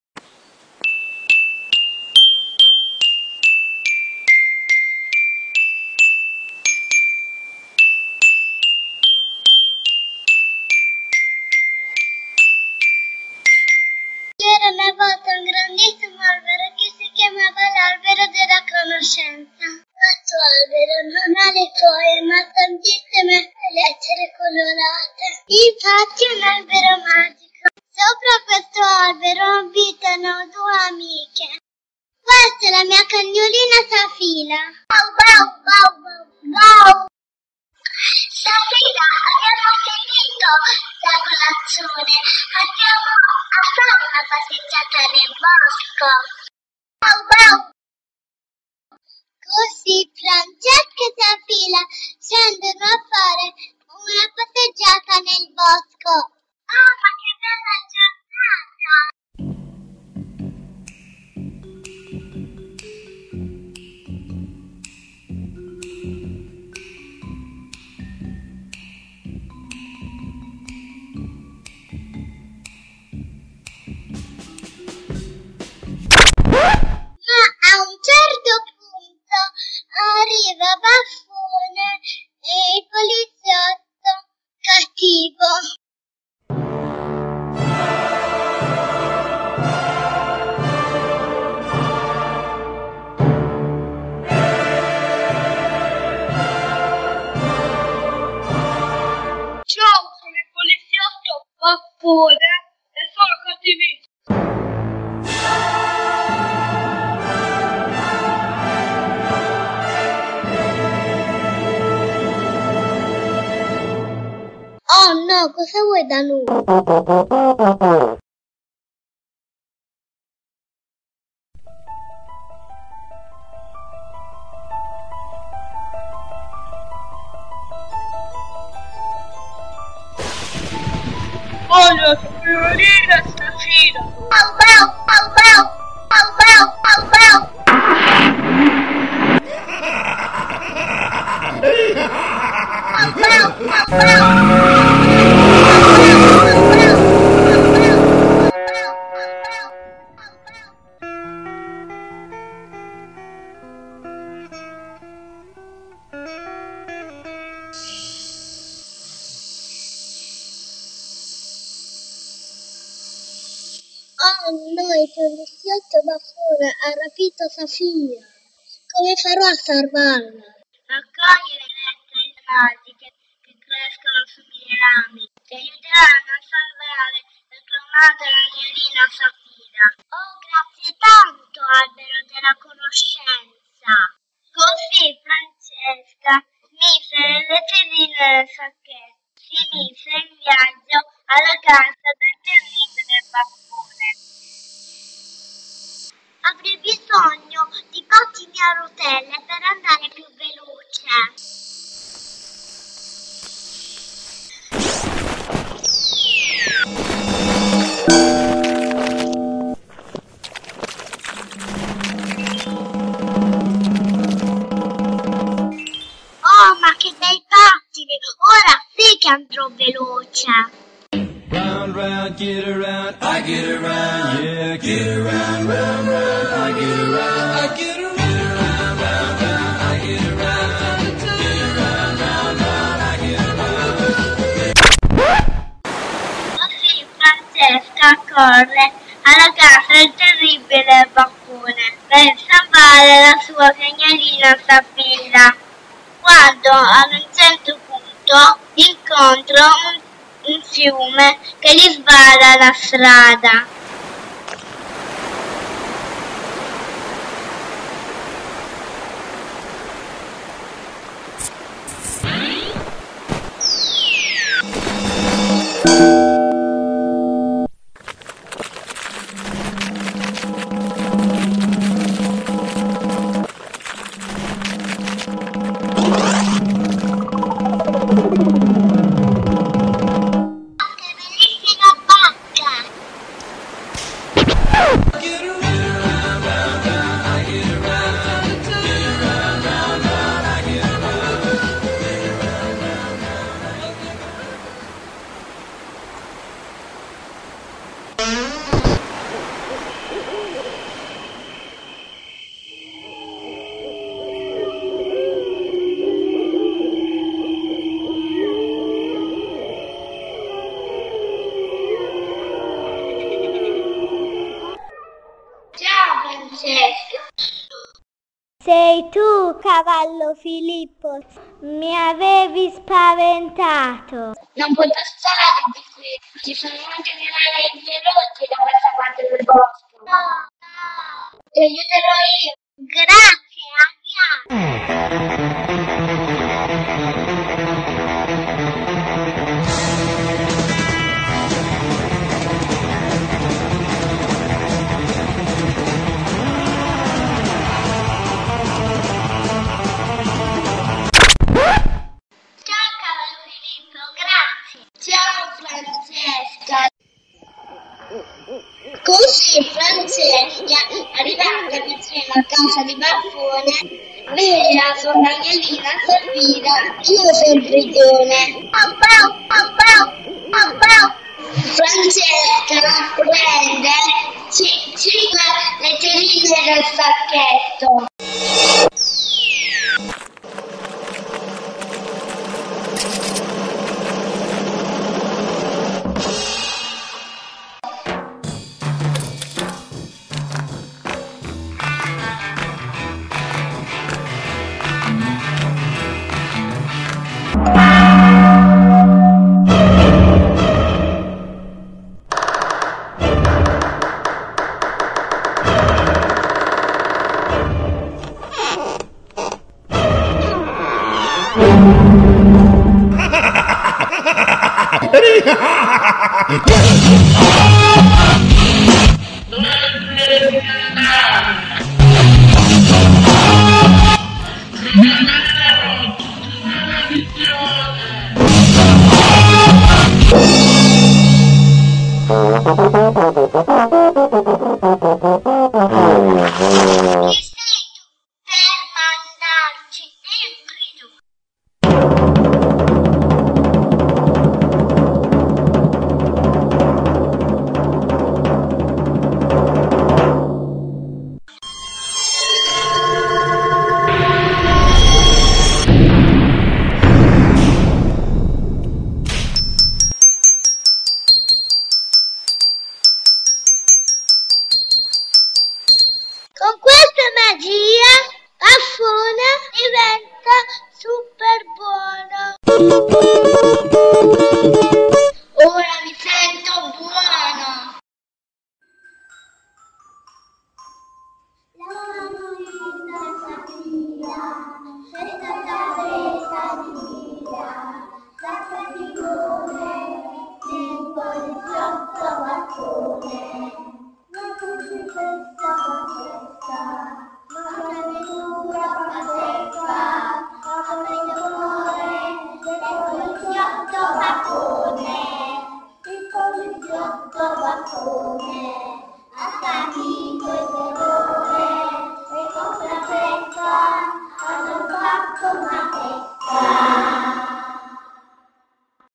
Francesca, Safira e il poliziotto Baffone
Fiaba inventata e disegnata dai bimbi della scuola comunale d' infanzia Principessa di Piemonte animazione regia montaggio Claudio Losghi Ranieri